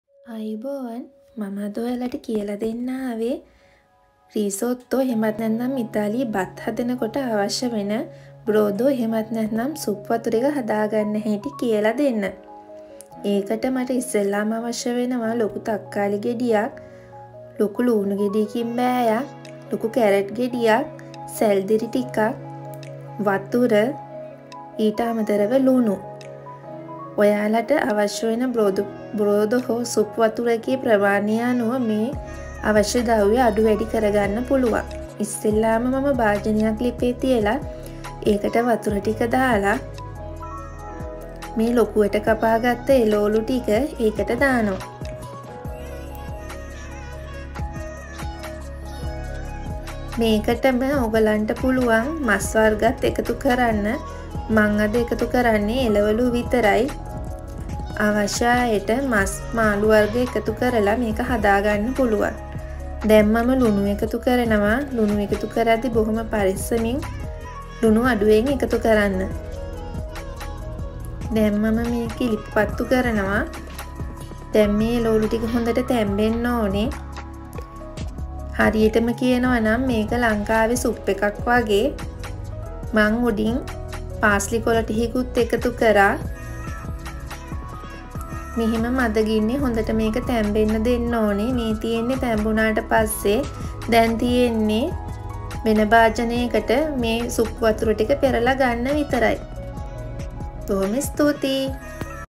Brodo (ඉතාලියේ බත් උයන සුප් වතුර ) episode 06
කැරට්
ළූනු
සැල්දරි
තක්කාලි
වතුර
අවශ්‍ය නම් මස් වර්ගයක්
පාස්ලි ටිකක්